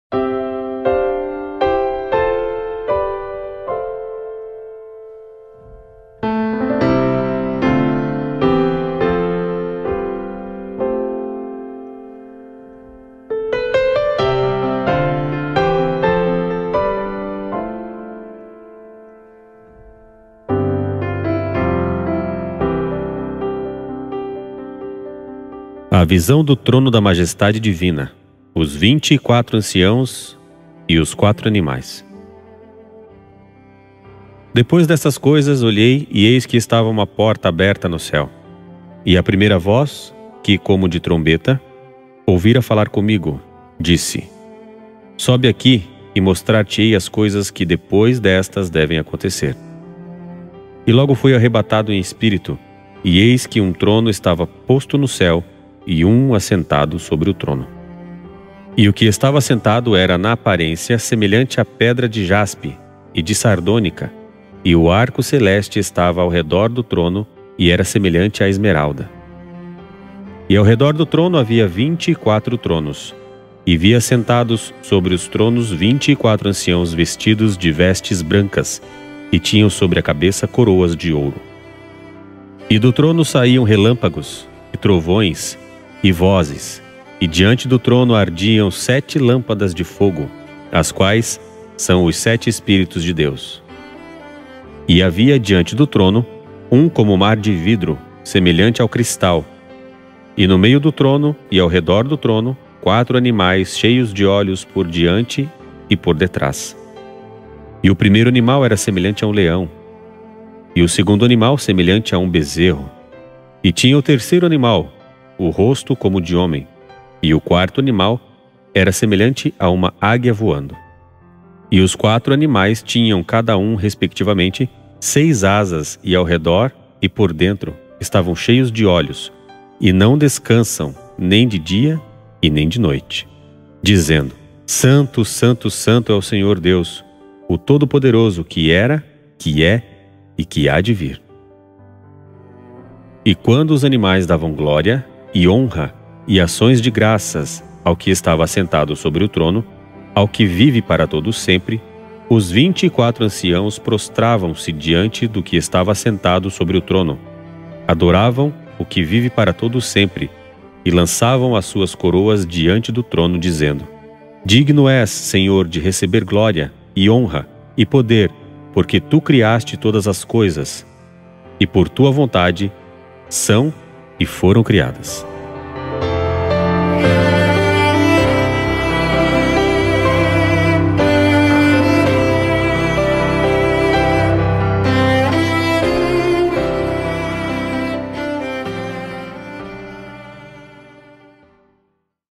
Glauber do Ouro - Apocalipse 4
Estou com o propósito de gravar o livro de Apocalipse todo. Postarei aqui no canal 1capítulo por semana! Vem comigo!